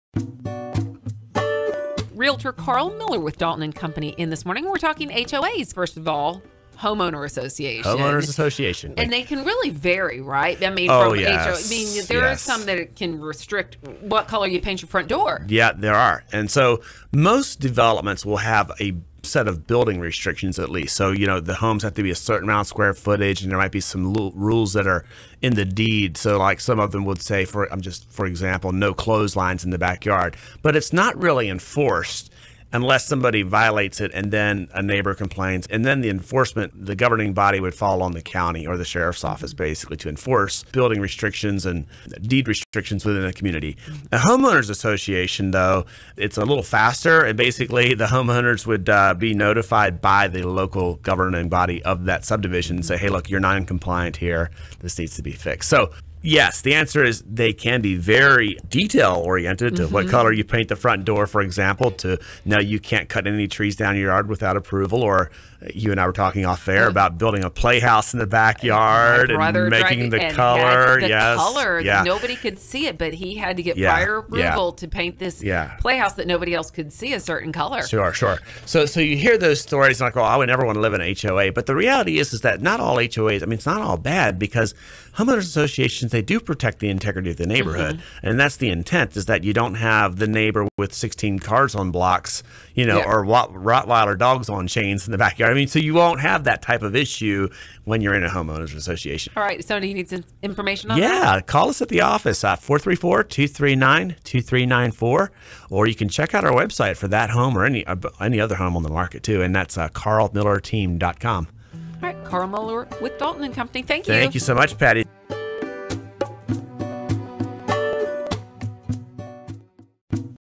Karl Miller Team Real Estate Tips for 2018 Home owner associations
Karl Miller of Karl Miller Team REALTORS in Lynchburg VA discusses Home owner associations and what you need to find out about a neighborhood's do and don't clauses before buying your home. Learning about the fine print before buying can make a big difference about your peace of mind living in your new home.